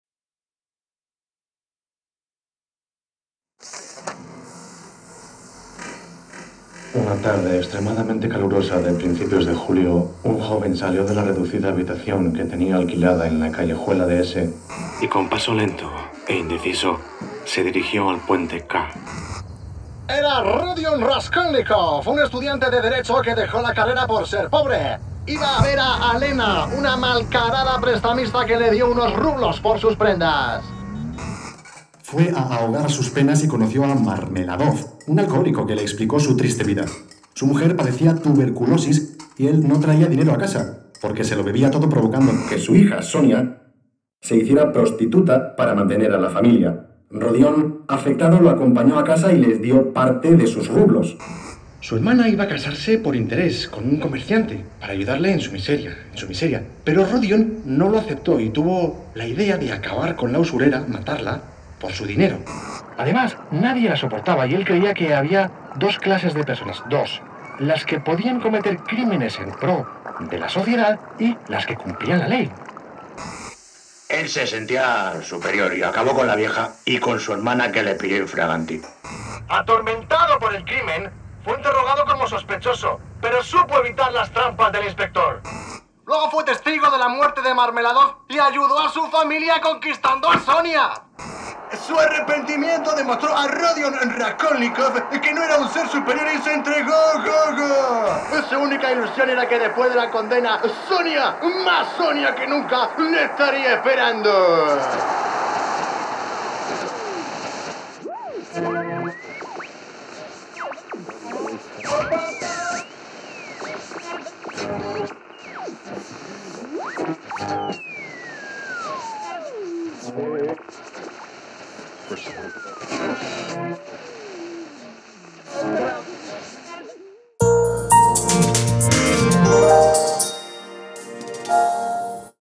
Nº20 Crimen y castigo - Grandes Obras Universales
Rodion Romanovich Raskolnikov es un joven estudiante que oprimido por la pobreza se deja llevar por un plan febril acorralando aún más en su soledad e ira contenida. Comete un crimen, y a partir de ese momento su vida está marcada.